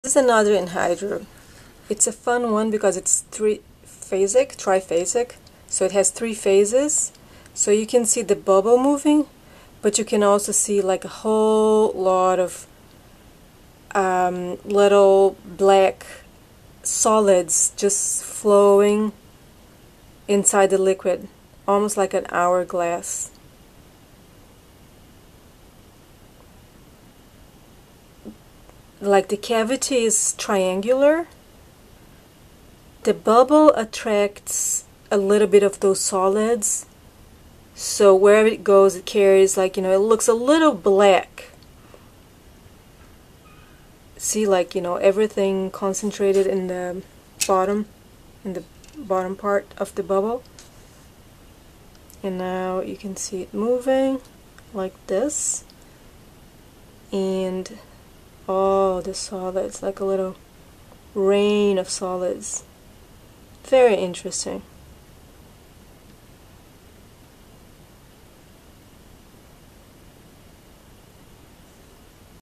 Quartz with Triphasic Inclusions
A beautiful 23.90 carats Quartz with three phases of inclusions: gas, liquid and solid. The solids are tiny specks of Graphite. Isn't it just amazing to think that this water has been trapped there for millions of years?